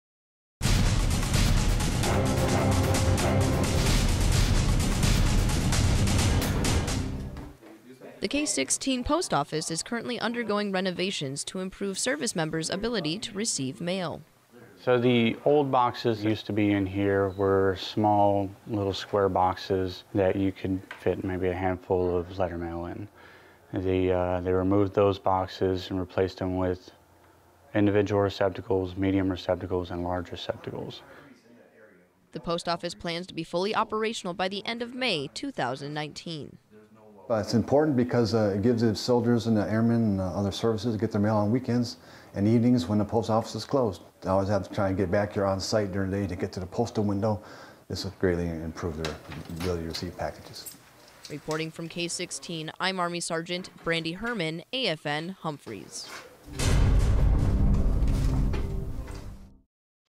AFN Humphreys - K-16 Mail Room
The K-16 Post Office is currently undergoing renovations to improve day-to-day operations.